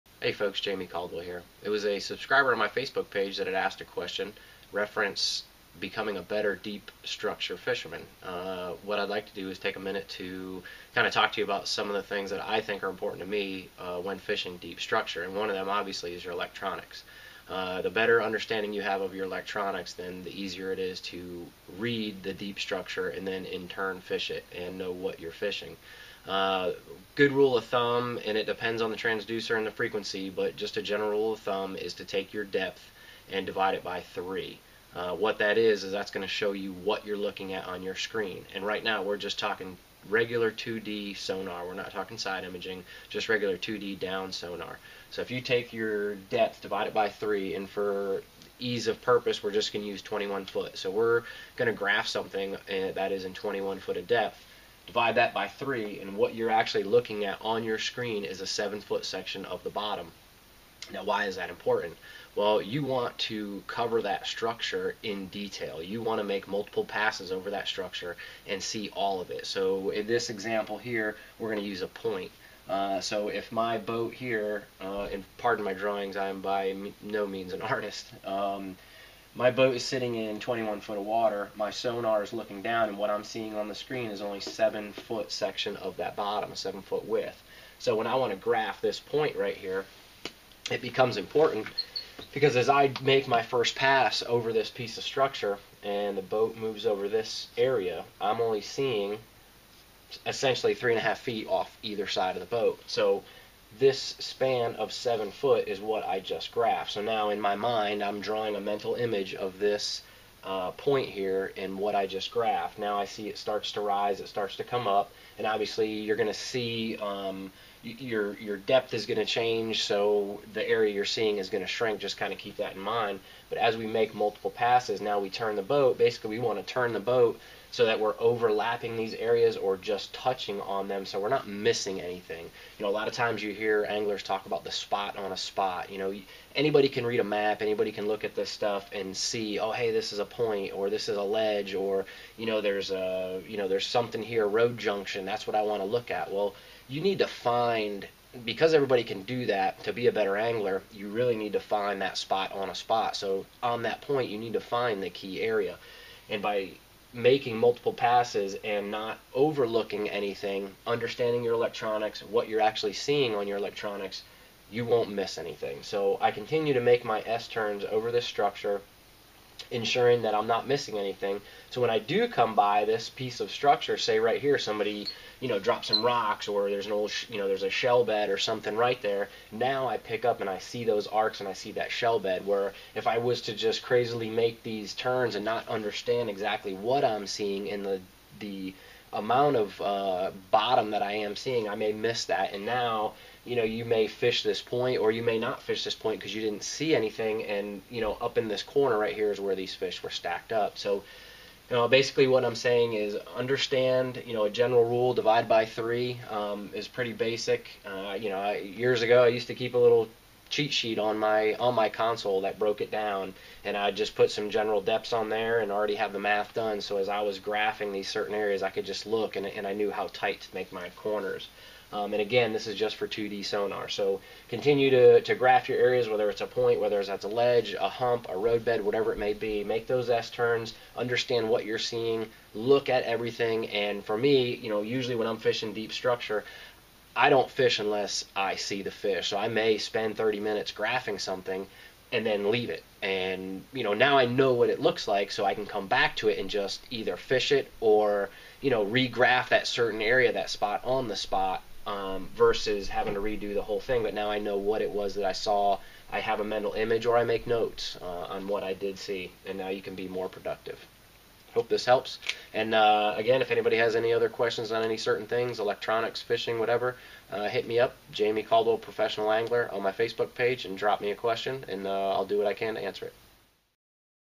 Deep structure electronics class
Class on using your electronics to graph deep structure. The focus is on better understanding your 2D sonar returns and tips on graphing.